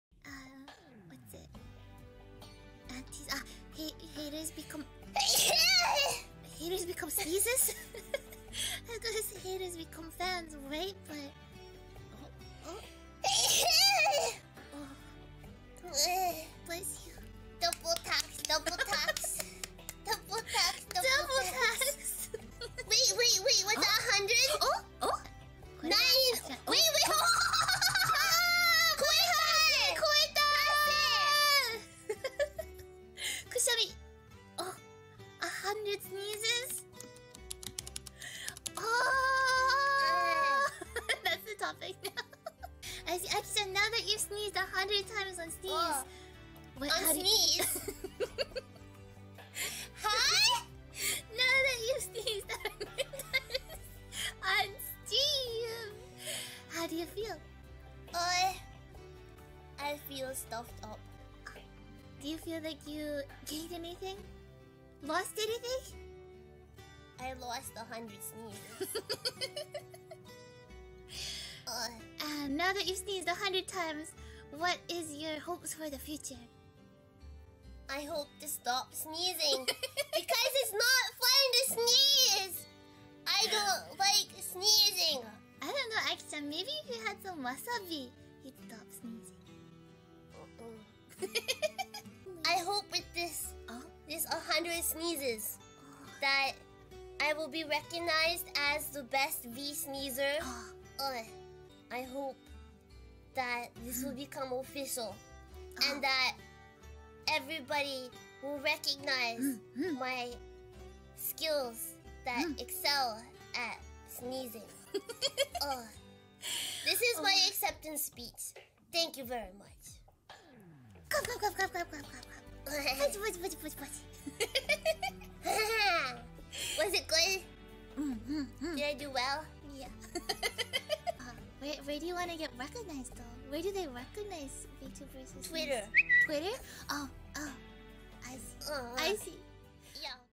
Akira declares herself as the #1 VSneezer after sneezing 100 times on stream
I think we can confidently say that she's the best. She blew the rest of the competition away